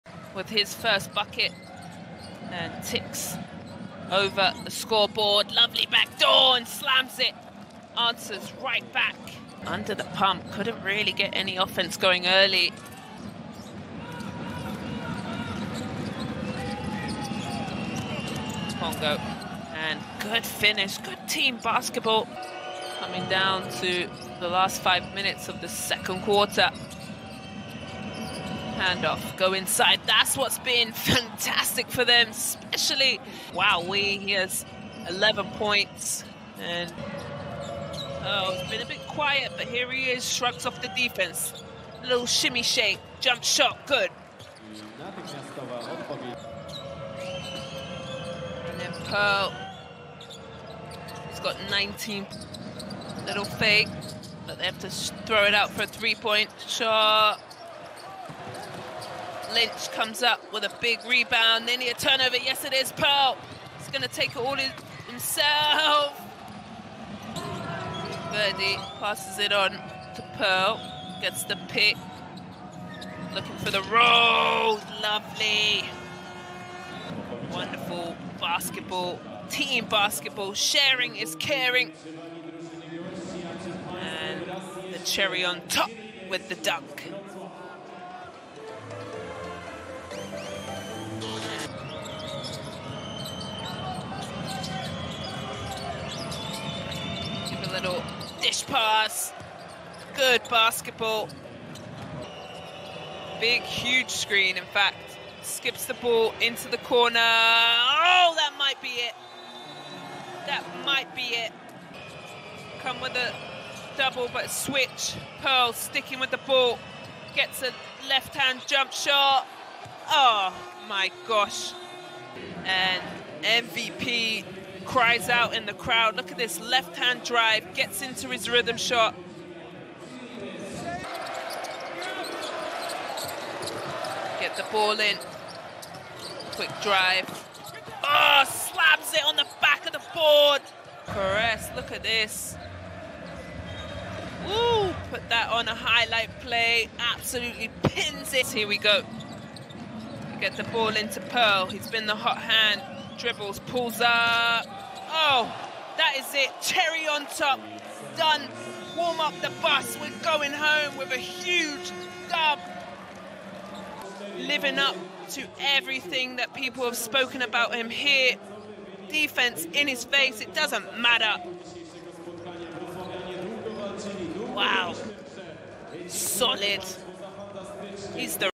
ÖSSZEFOGLALÓ || WKS Slask Wroclaw - Falco-Vulcano Szombathely // Bajnokok Ligája F-csoport, 1. meccs
Kövess minket!